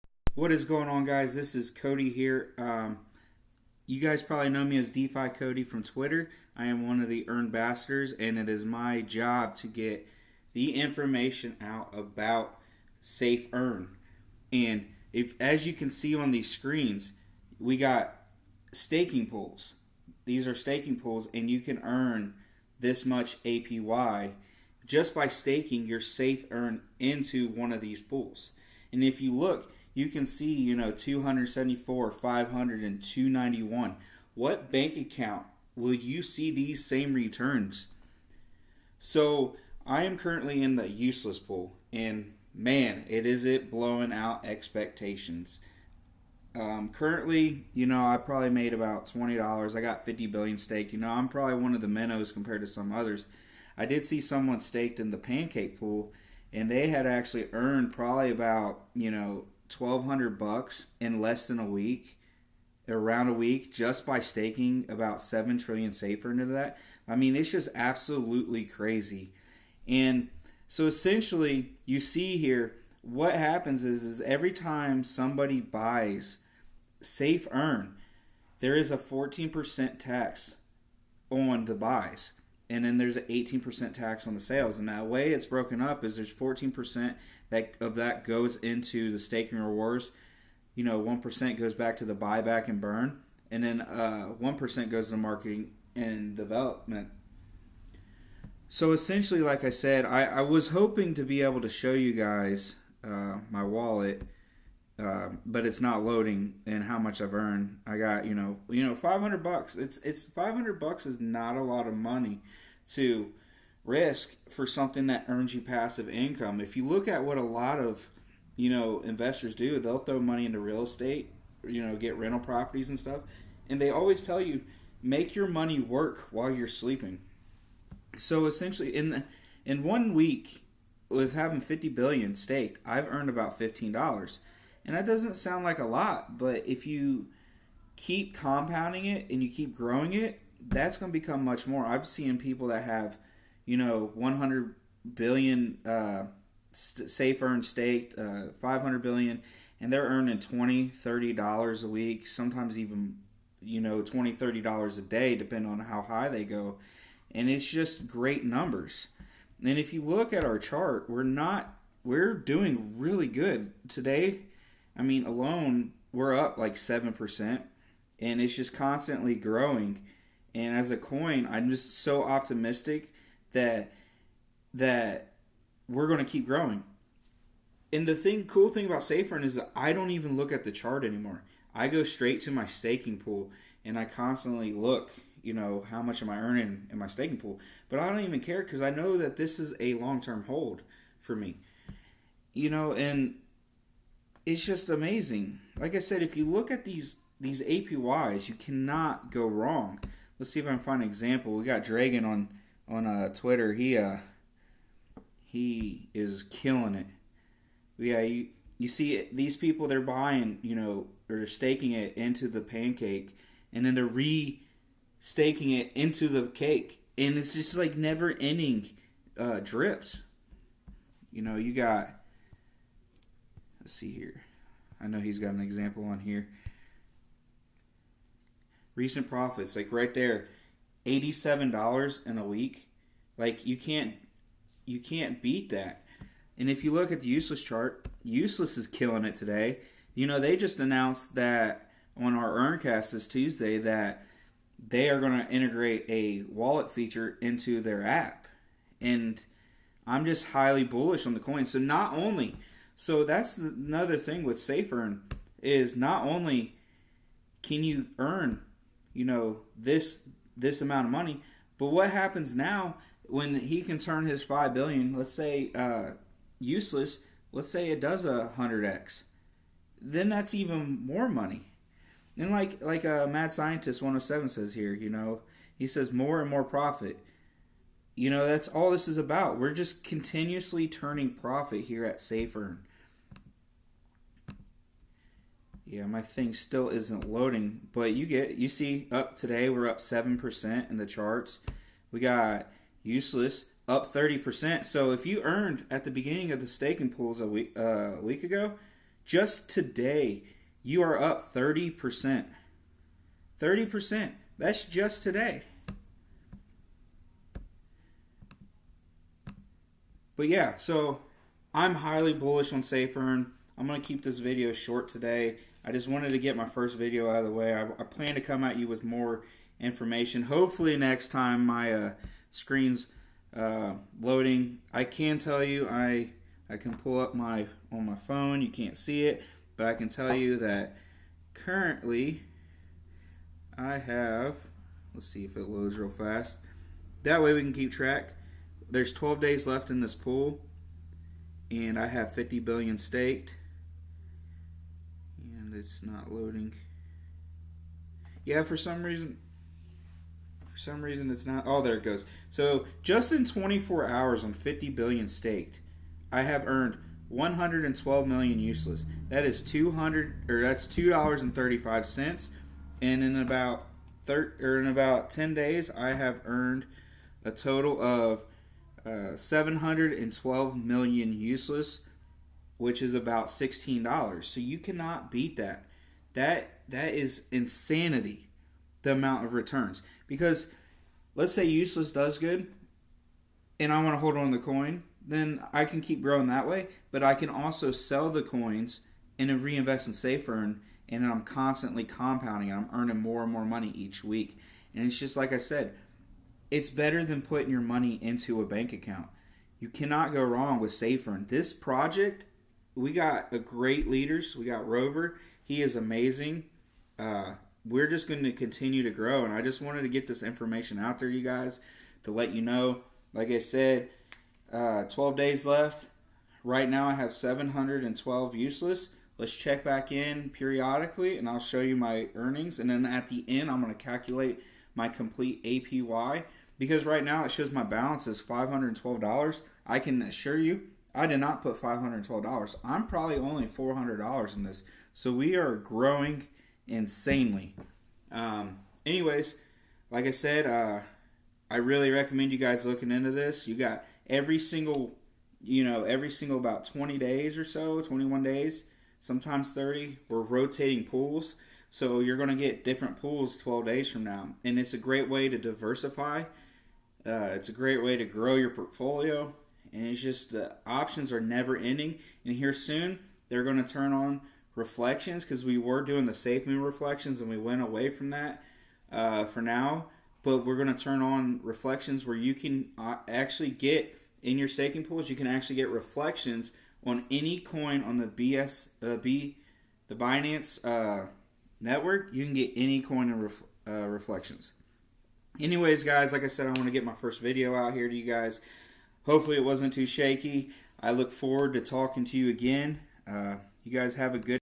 Earn passive income and free tokens with Safeearn staking pools!!!
Today i talk about the staking pools with Safeearn soon to be earn hub.
i also talk about useless one of my free coins